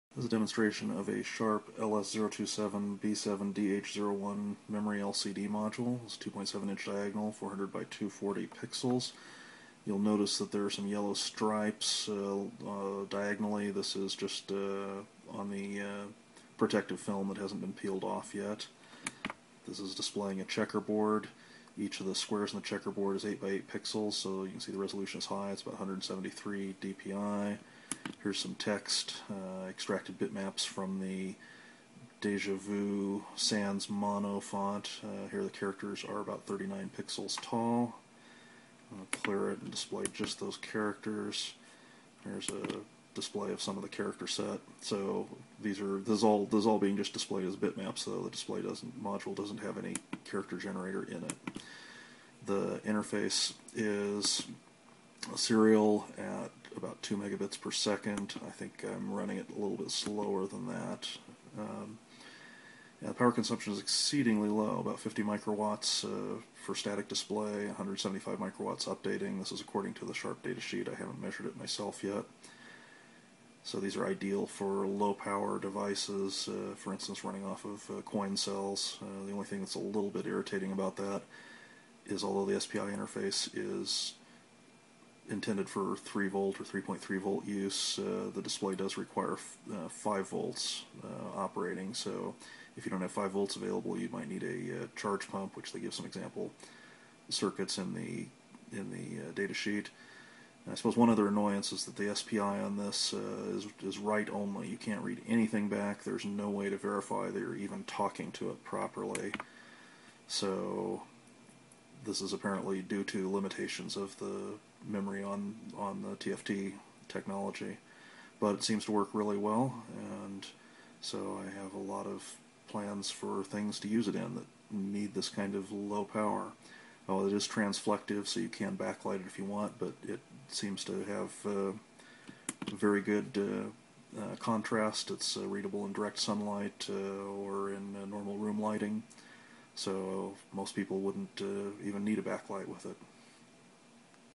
Sharp 2.7" memory LCD demo
Demo of Sharp LS027B7DH01 2.7" memory LCD. The display has 400x240 pixels, and operates on extremely low power, 50 μW for a static display, and average 175 μW during updates. It is ideal for use in battery-powered devices, including those powered by lithium coin cells.